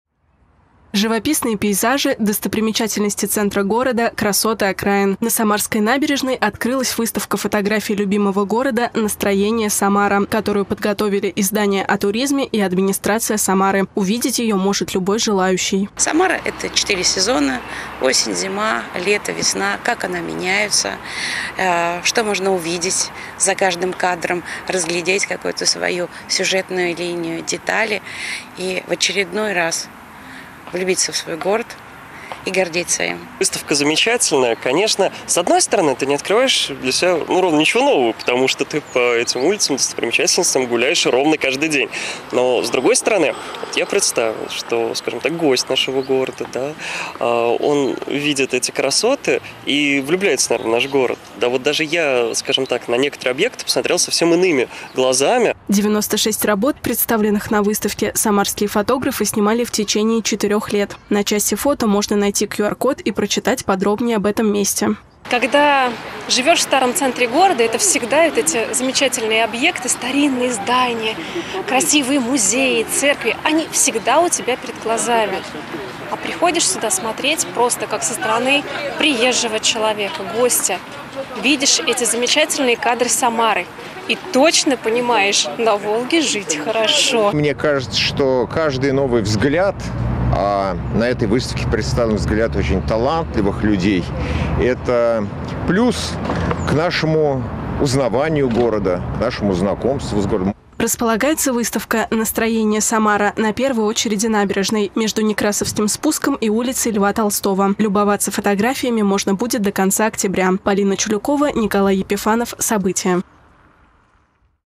На набережной открыли фотовыставку "Настроение — Самара"
Взглянуть на город по-новому. В столице региона открылась выставка "Настроение - Самара". Около сотни фотографий города на Волге радуют глаз горожан и гостей города. Что запечатлели фотографы и как долго  продлится работа экспорзиции,  расскажет Полина Чулюкова.